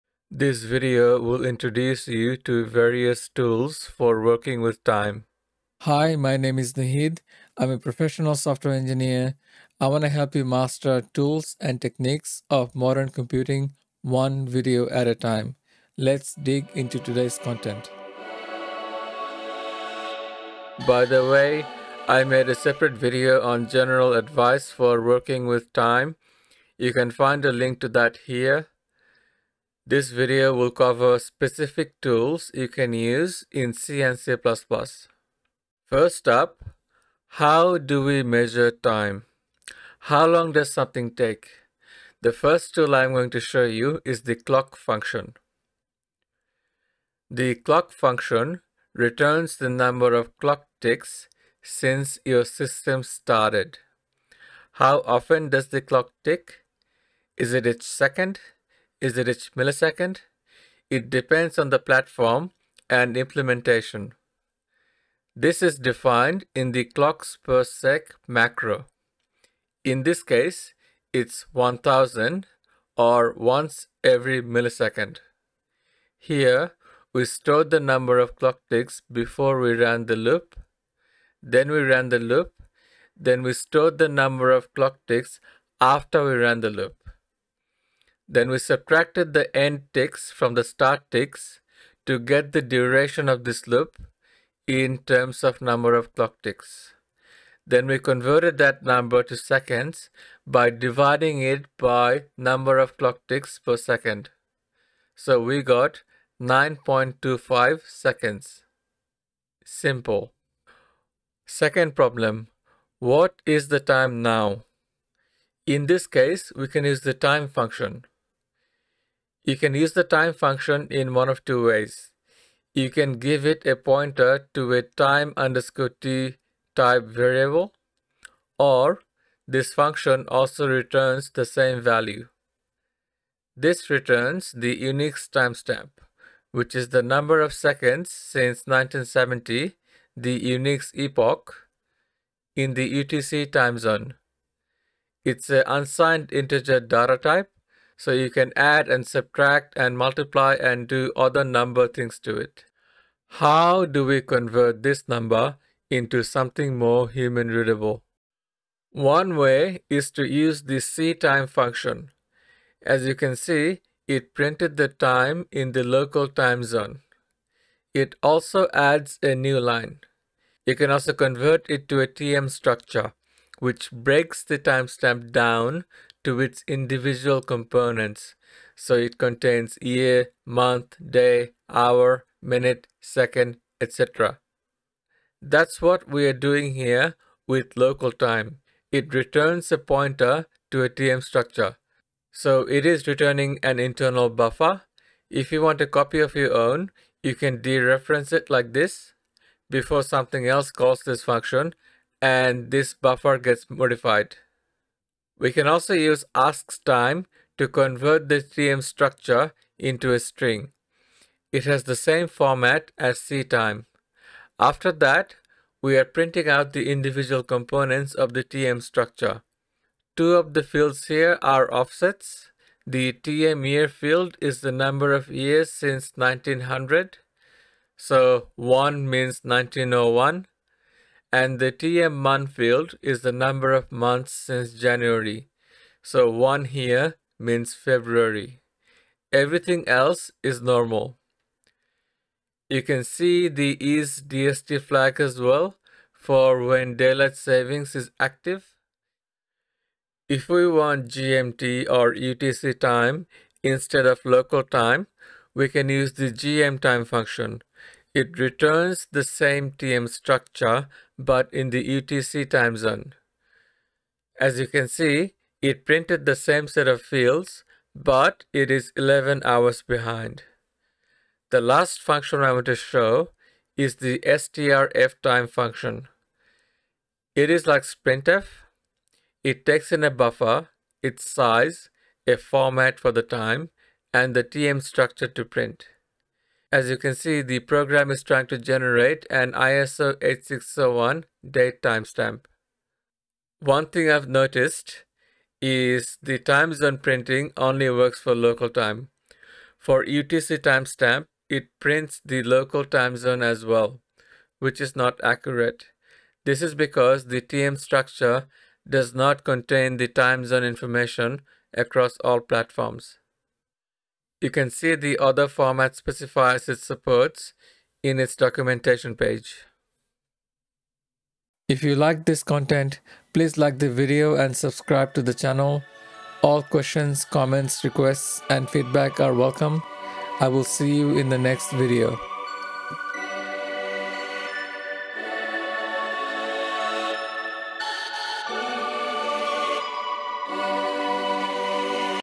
How to work with dates and times in C/C++ using ctime? [1.42]
How to work with dates and times in C/C++ using ctime?

00:00 - Intro
00:35 - How to measure time using the clock function?
01:50 - How to get the current time?
02:33 - How to convert unix timestamp to human readable local time?
02:50 - How to decompose an unix timestamp into year, month, day, hour, minute, second?
03:24 - How to convert a tm structure into human readable string?
03:34 - How to print tm structure fields manually?
04:09 - How to get GMT tm structure instead of local time?
04:31 - How to output formatted date time using strftime?